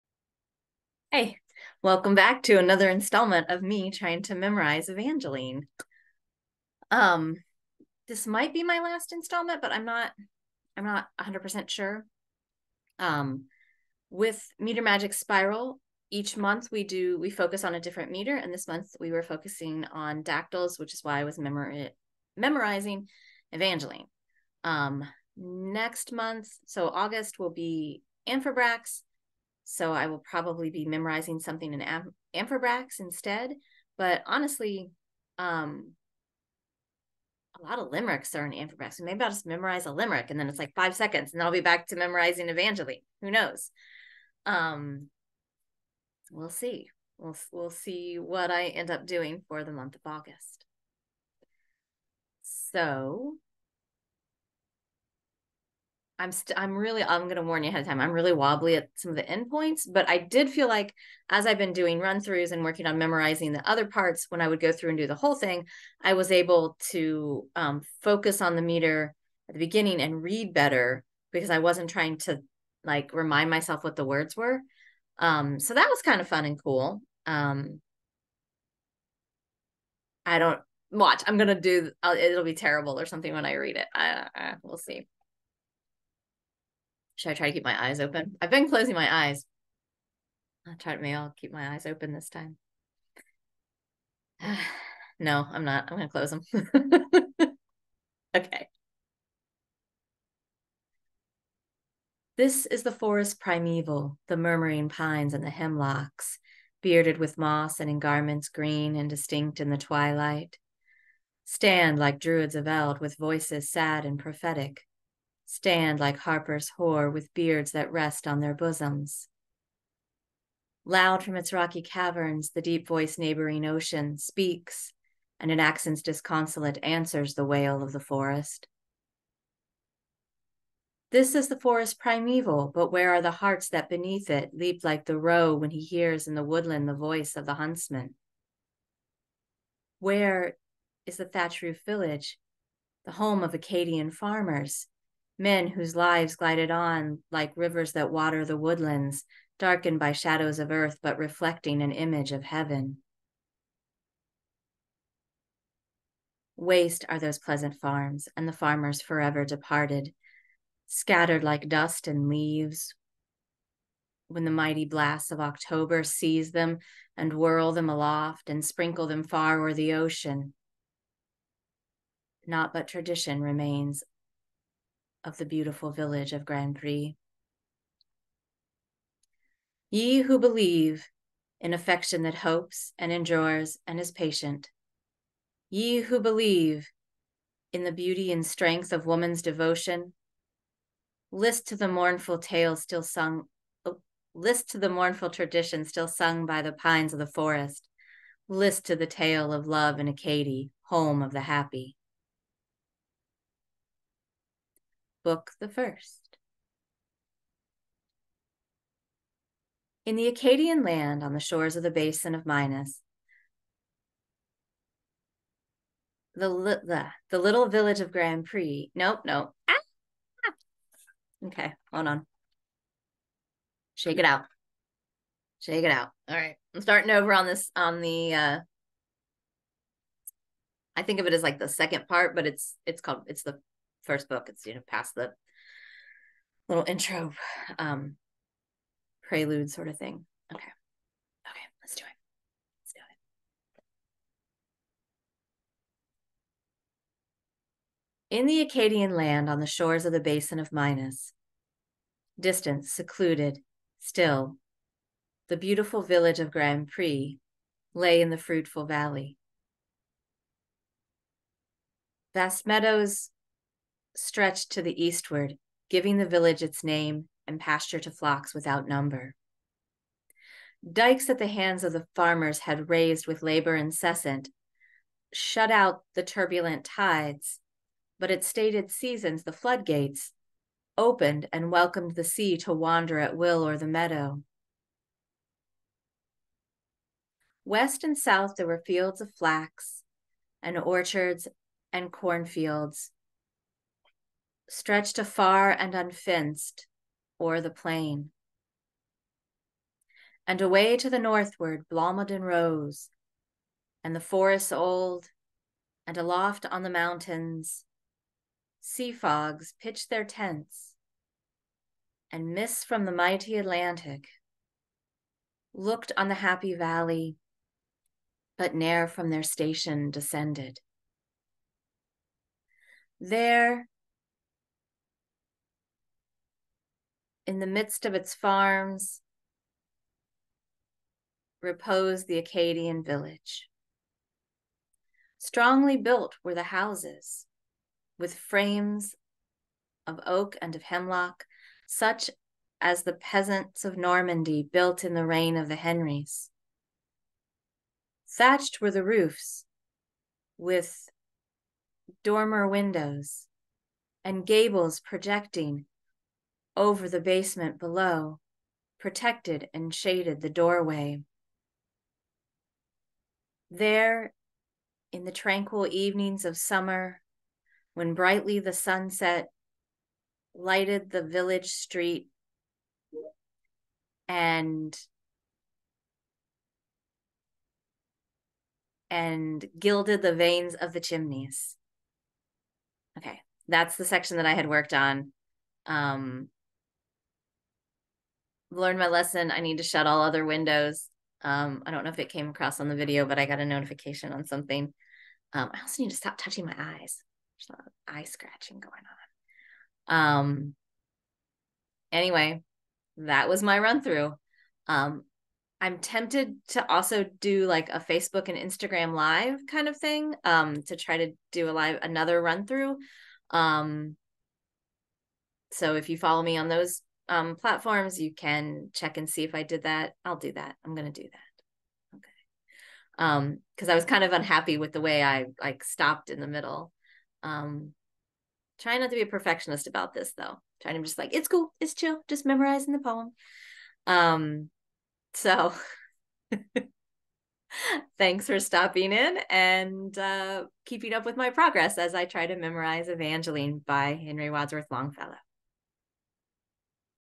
Memorizing Evangeline Week 3
Continuing to try and memorize Evangeline by Henry Wadsworth Longfellow as part of the Meter Magic Spiral with @AnnieFinchPoet and the@poetcraft ! It didn't go great this week because my last EMDR session started playing havoc with my brain. Posting for the realness of it all.